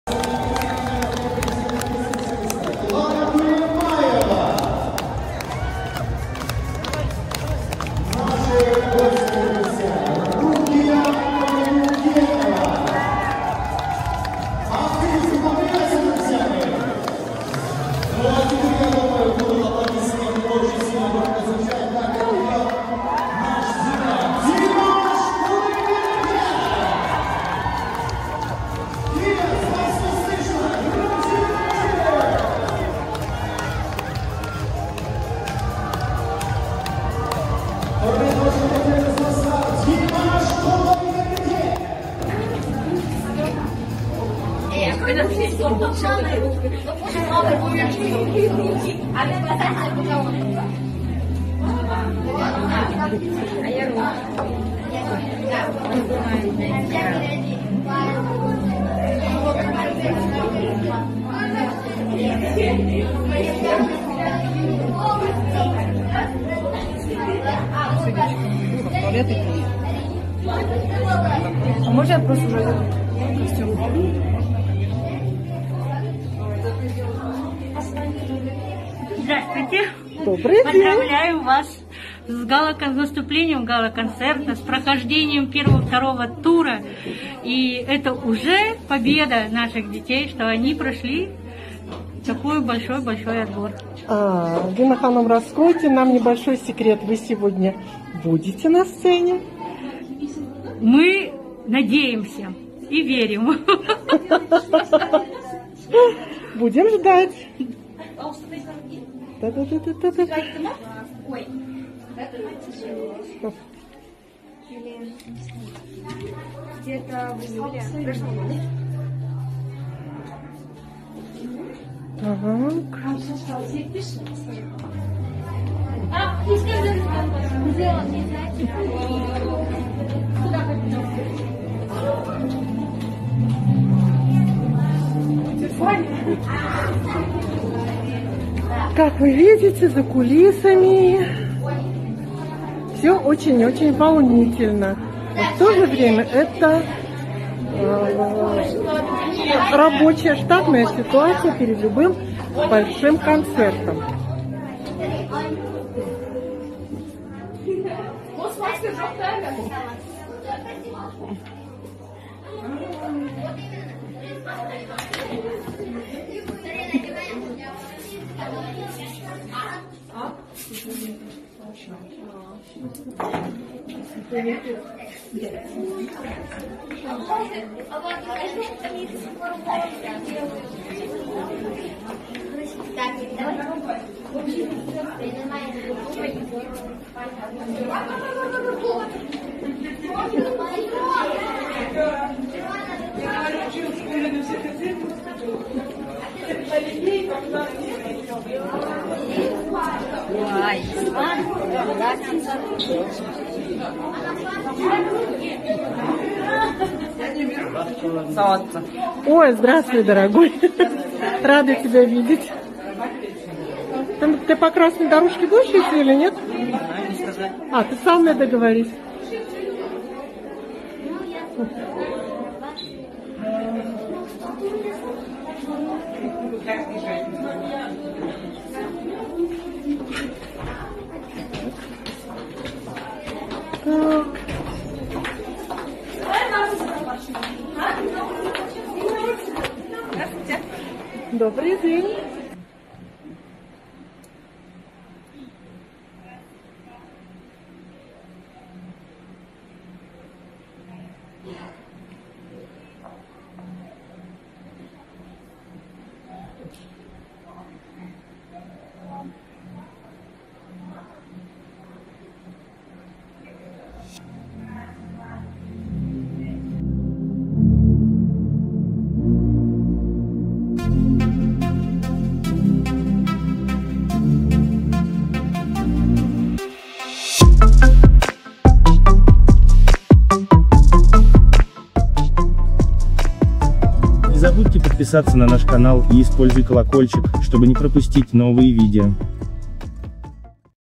📣 ДИМАШ КУДАЙБЕРГЕН на гала-концерте в АКТОБЕ Подготовка и начало концерта ✯SUB✯
Гала-концерта «BAQYTTY BALA» 2021
Последняя репетиция перед Гала концертом Baqytty Bala 2021 и начало концерта
Спасибо огромное!